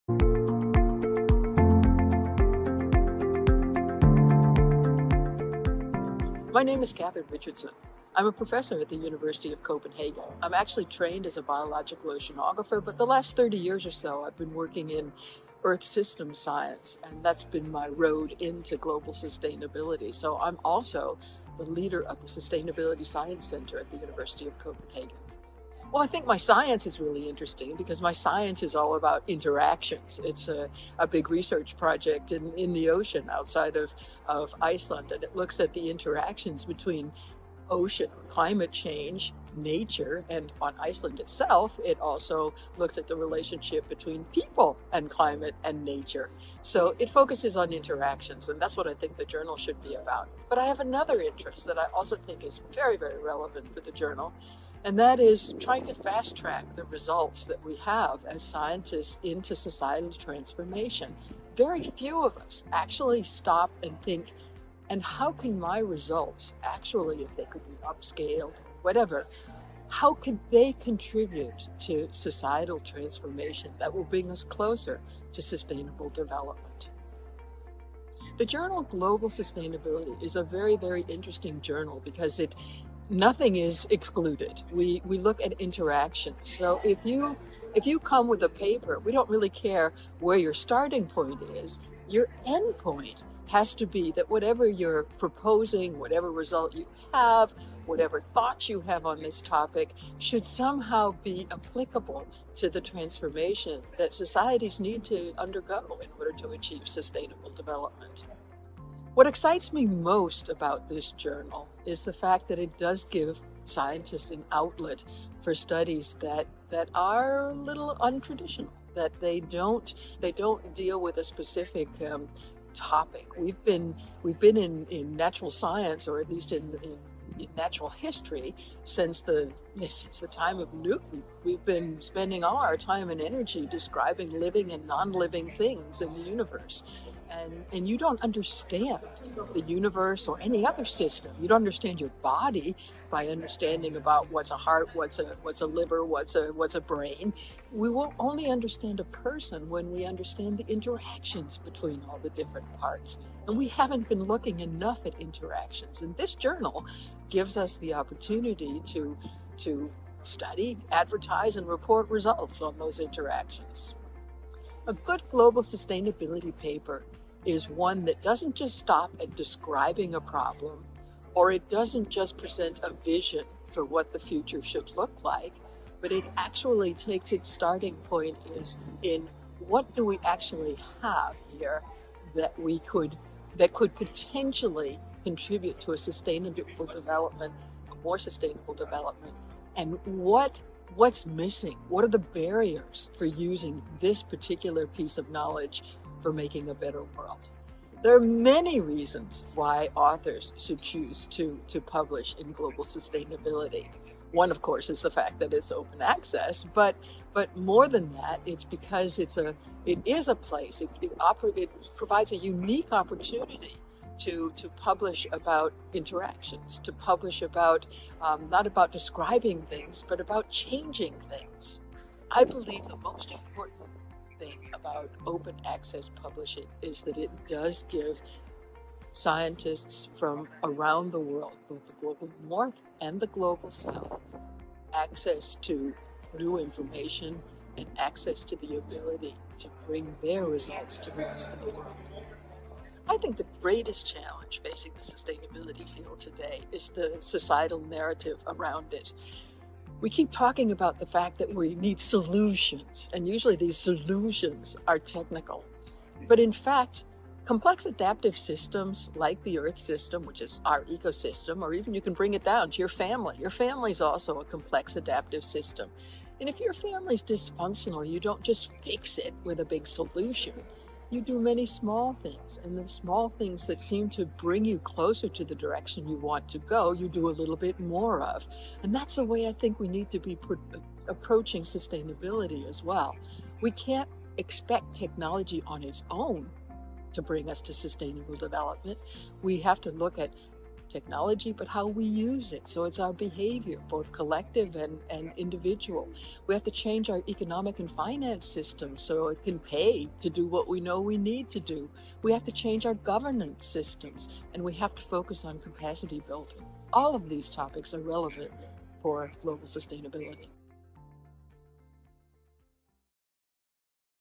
Katherine Richardson
An insight into Global Sustainability with Advisory Member, Katherine Richardson